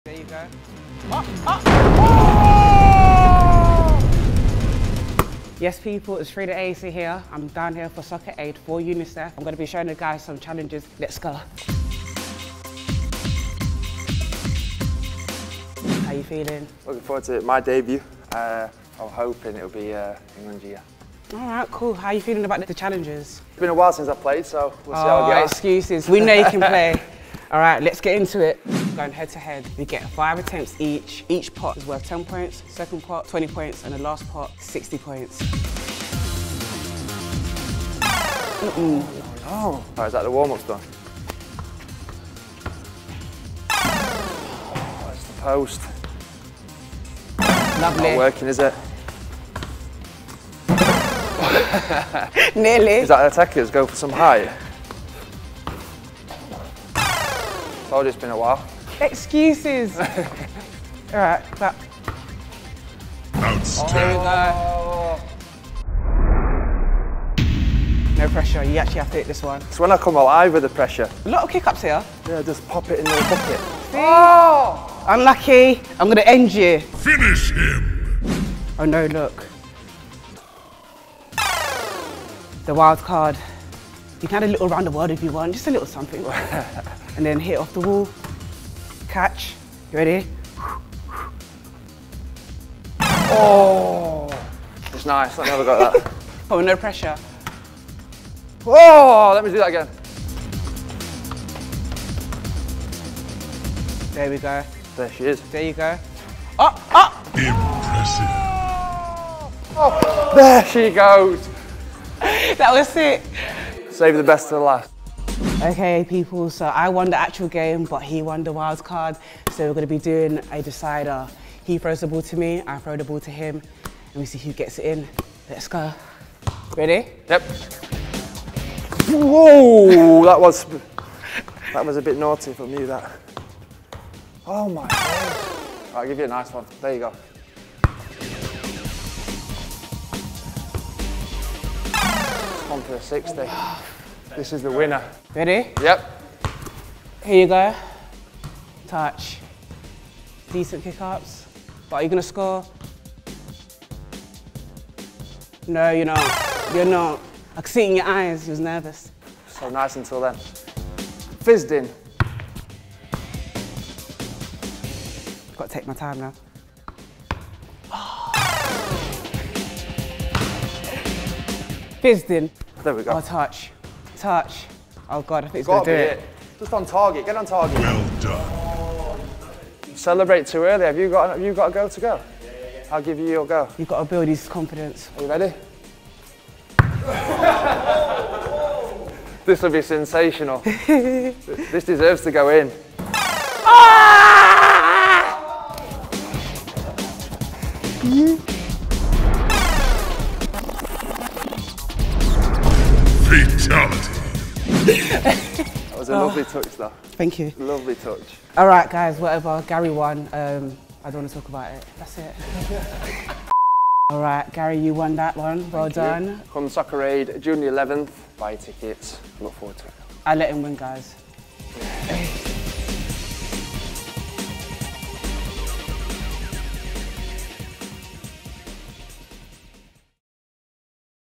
NO WAY GARY CAHILL DID THAT 🤯 | CHELSEA LEGEND'S CRAZY SKILL
Champions League winner Gary Cahill takes on the Top of the Pots challenge with pro baller Freda Ayisi for Soccer Aid 2023.

For more information and to get your tickets visit socceraid.org.uk.